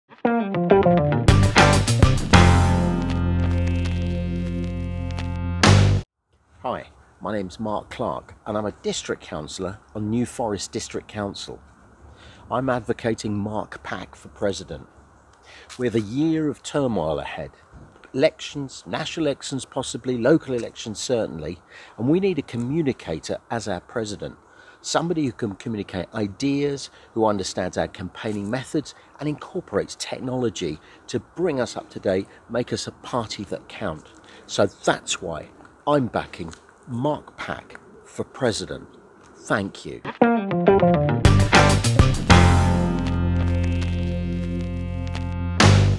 He'll use technology to bring us up to date - Cllr Mark Clark is backing Mark Pack
Hear why Cllr Mark Clark is supporting Mark Pack to be the next President of the Liberal Democrats.